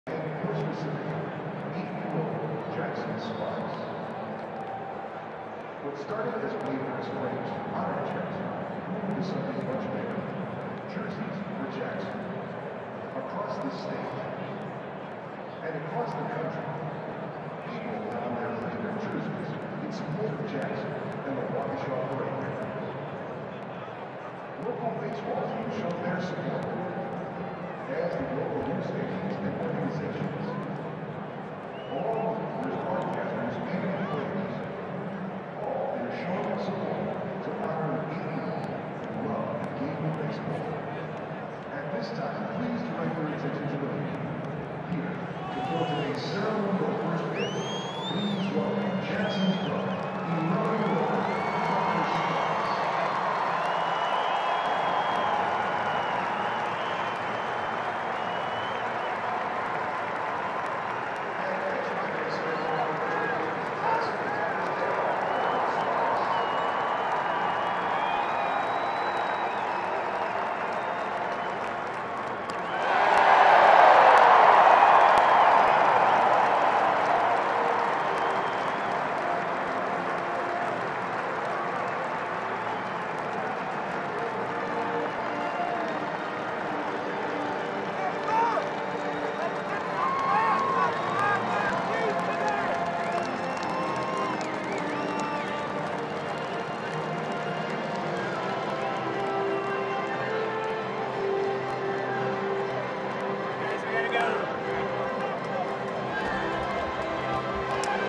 Video: Brewers Opening Day first pitch
Brewers Opening Day first pitch: Tucker Sparks, 12, is the brother of Waukesha parade victim Jackson Sparks. He had the honor of throwing out the first pitch at Brewers Opening Day!